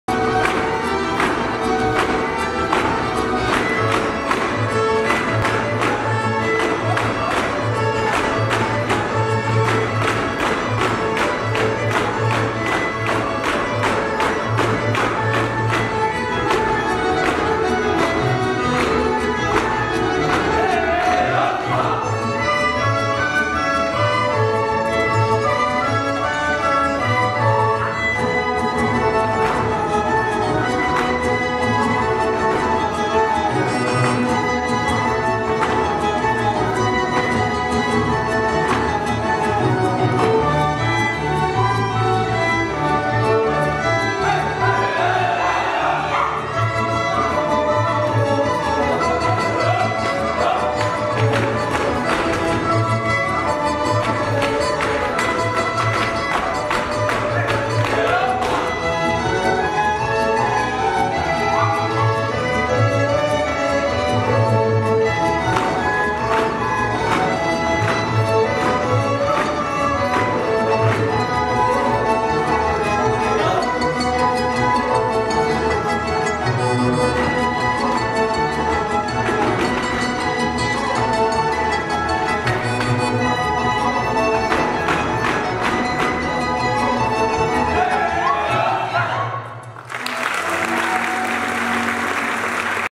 КONCERT AКUD “DIMITRIJE КOTUROVIĆ“ "IZNAD IGRE"
Nisam siguran kako se zove ples ali izvodjaci su sjajni u svakom slucaju.